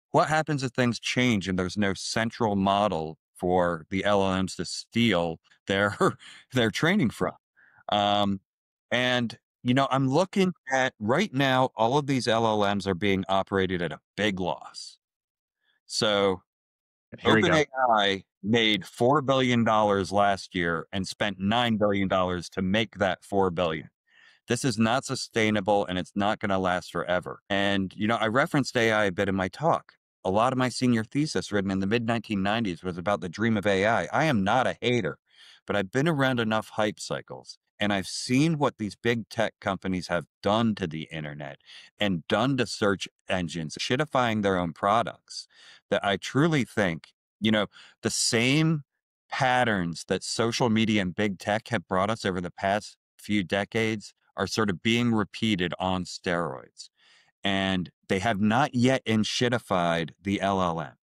LLMs: Peak or Bust?
Tim Allen dives into the shaky underlying financials of leading AI models. A clip from an upcoming episode on "AI in the Real World."